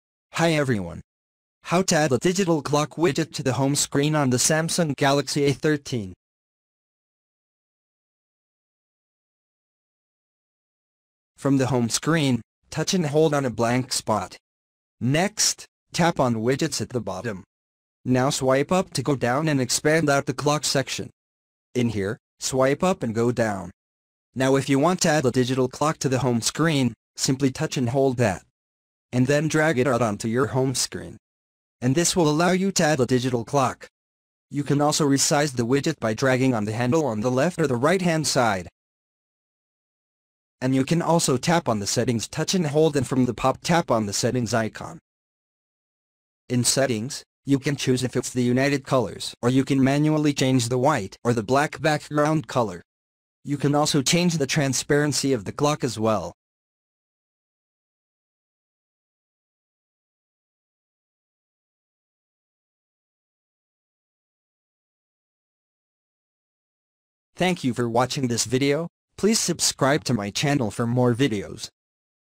Samsung Galaxy A13: How to Add a Digital Clock Widget to the Home Screen
How to Add a Digital Clock Widget to the Home Screen on Samsung Galaxy A13.